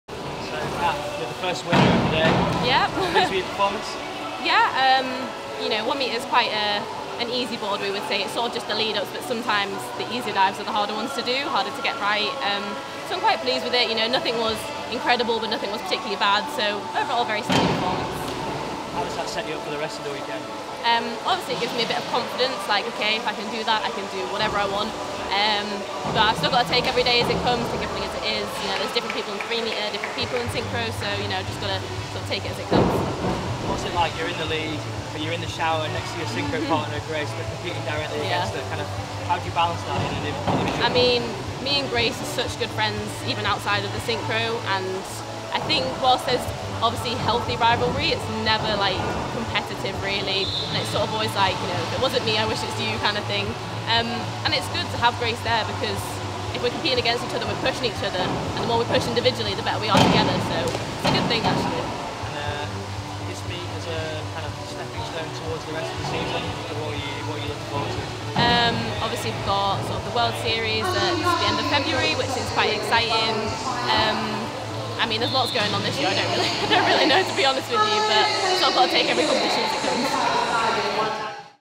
Kat Torrance - Women's 1m British National Diving Cup winner 2019
We caught up with Leeds diver Kat Torrance after she claimed the opening title of the British National Diving Cup 2019 in Plymouth in the Women's 1m Springboard.